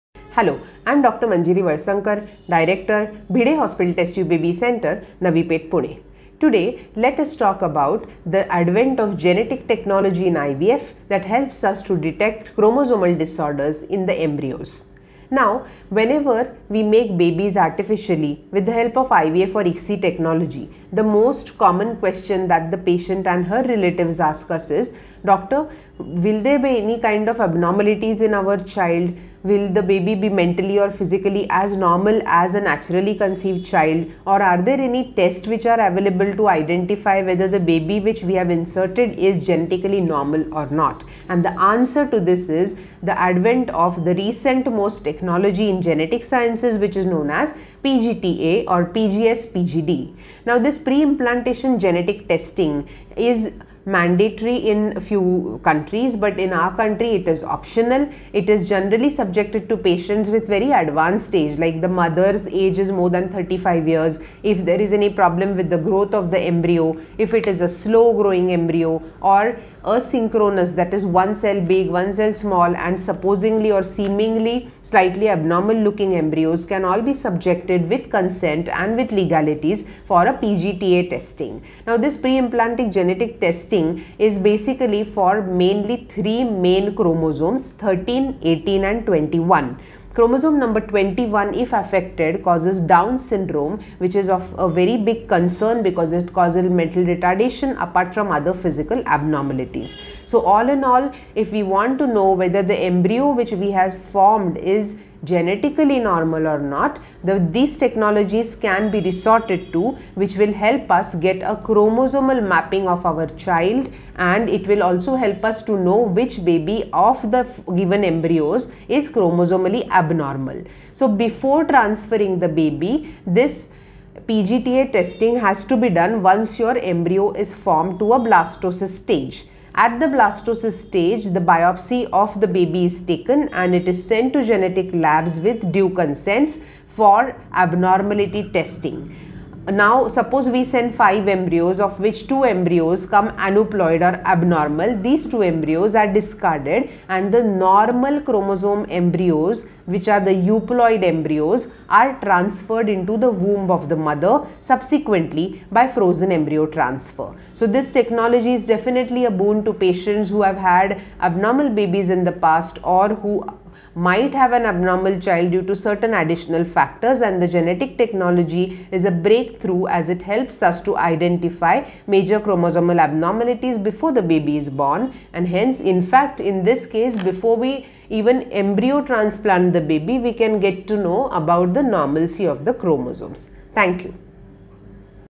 Advanced technology PGTA PGS to identify chromosomal abnormalities in embryo in IVF ICSI
PGTA PGS to identify chromosomal disorders  in embryos
Dr Manjiri Valsangkar Pune
Bhide hospital IVFcentre Pune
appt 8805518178
opd 12 to 5 pm